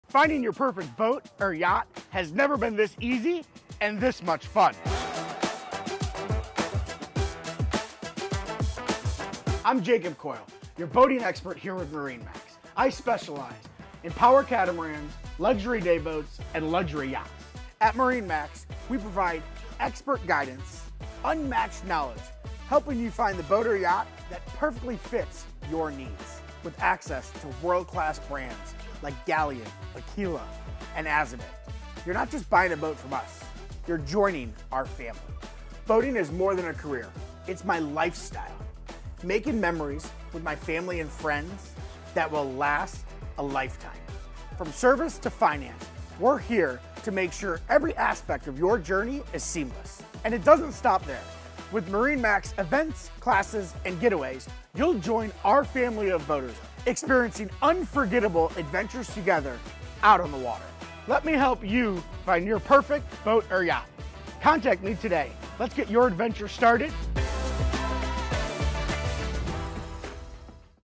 Discover Your Perfect Boat or Yacht with Jacob Coyle
Looking for your next boat or luxury yacht? Jacob Coyle is your go-to resource at MarineMax! With nearly a decade of experience, Jacob knows exactly what it takes to match clients with the perfect boat for their lifestyle. Specializing in Power Catamarans and luxury yachts, he represents top brands like Aquila, Aviara, Sea Ray, Boston Whaler, Saxdor, Galeon, and Azimut—all known for their exceptional craftsmanship and performance on the water.

Jacob’s passion is helping people experience the thrill of boating, and his commitment to providing top-tier service makes him a trusted partner for clients across North America. From first-time boaters to seasoned yacht owners, Jacob has the expertise and dedication to ensure a seamless, enjoyable buying experience.

Let Jacob guide you to your dream boat or yacht—reach out today to start your journey with MarineMax!